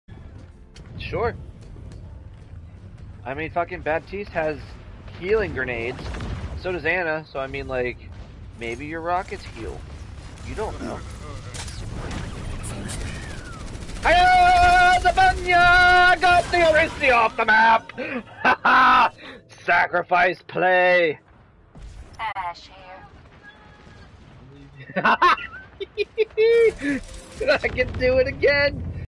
Hammy the Noble
Sometimes, you gotta make that sacrifice play. A noble act from the brilliant hamster -- Watch live